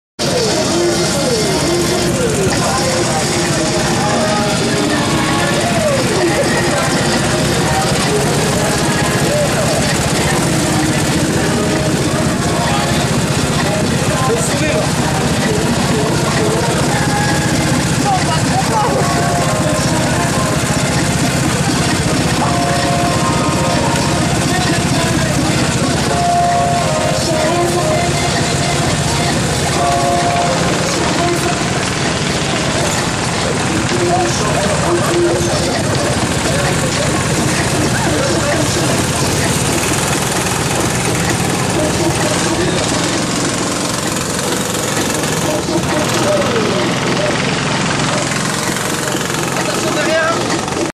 Départ session Espoey
Filles en session libre volant GoKart Honda 190CM3 4t" Stearing" parfait" Krank" complet, freinages avec un coeur gros comme ça! Rien à dire "accurency target" Black and white flag at last. Merci Espoey...bon wk Bye BAOOB2 A+